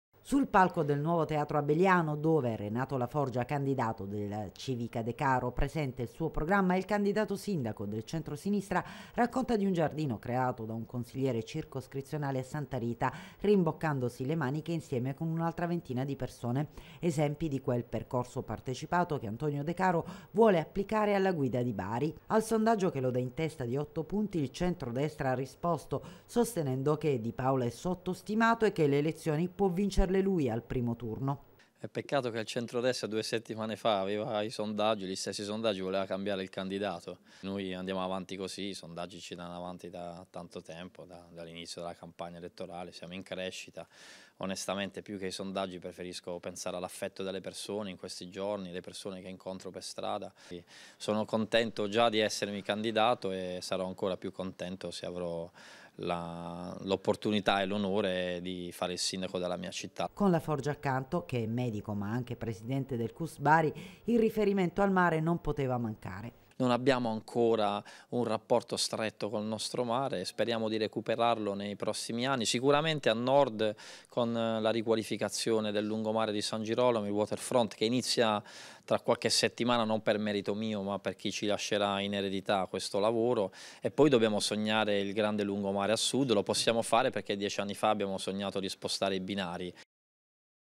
BARI DE CARO ALL' ABELIANO
APPUNTAMENTO ELETTORALE A BARI ANCHE PER IL CANDIDATO SINDACO DEL CENTROSINISTRA ANTONIO DECARO CHE IN UN INCONTRO PUBBLICO HA ESOSTATO GLI ELETTORI A D ANDARE A VOTARE IL PRIMMO 25 MAGGIO. E POI HA RILANCIATO LA QUESTIONE DEL FUTURO DEL LUNGOMARE CON UN CARDINE DEL PROPRIO PROGRAMMA.